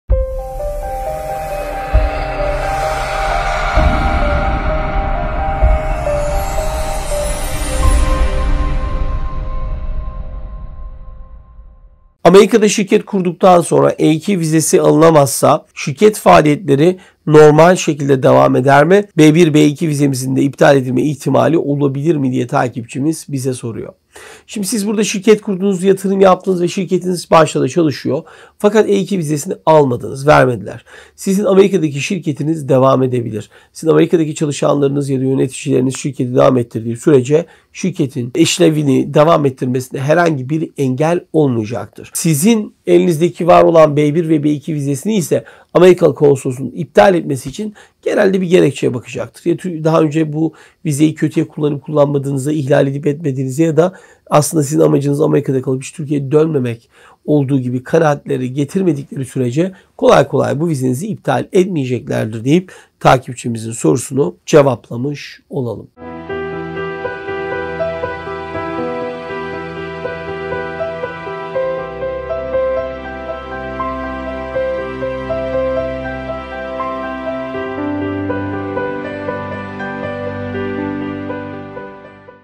AMERİKA’DA ŞİRKET KURUP E2 VİZESİ ALINAMAZSA ŞİRKETE NE OLUR ?
Soru: Amerika’da şirket kurduktan sonra E2 vizesi alınamazsa şirket faaliyetleri normal şekilde devam eder mi ? B1/B2 vizemizin de iptal edilme ihtimali olabilir mi ?